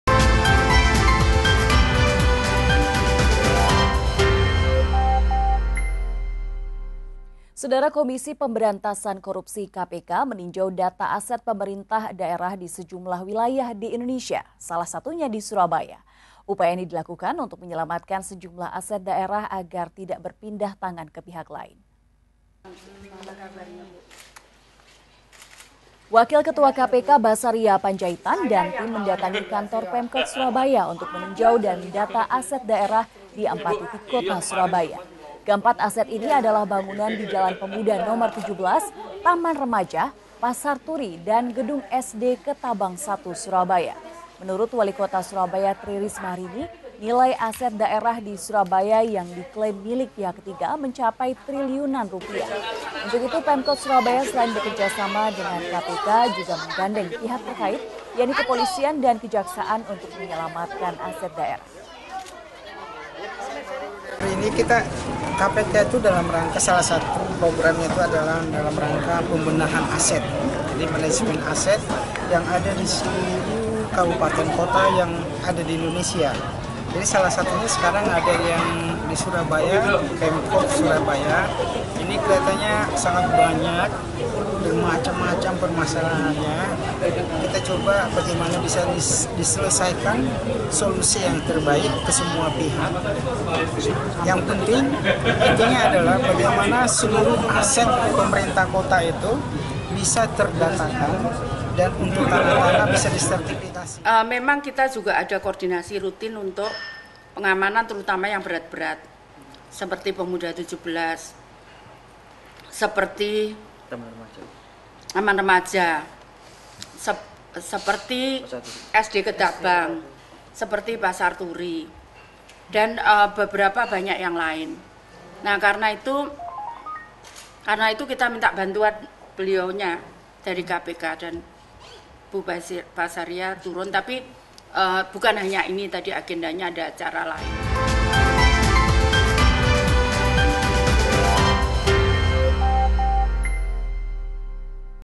UPAYA KPK DAN PEMKOT SURABAYA SELAMATKAN ASET DAERAH_KOMPASJATIM
KOMISI  PEMBERANTASAN  KORUPSI , K-P-K MENINJAU DATA  ASET  PEMERINTAH DAERAH , DI SEJUMLAH WILAYAH , DI INDONESIA,  SALAH  SATUNYA  ASET DAERAH , DI SURABAYA. UPAYA INI DILAKUKAN  PEMKOT SURABAYA , BERSAMA KPK UNTUK MENYELAMATKAN  SEJUMLAH ASET DAERAH , AGAR TIDAK BERPINDAH TANGAN  MENJADI MILIK PIHAK LAIN .

Jangan lupa untuk menyaksikan live streaming Kompas Jatim dan rangkaian news buletin lainnya dibawah ini :)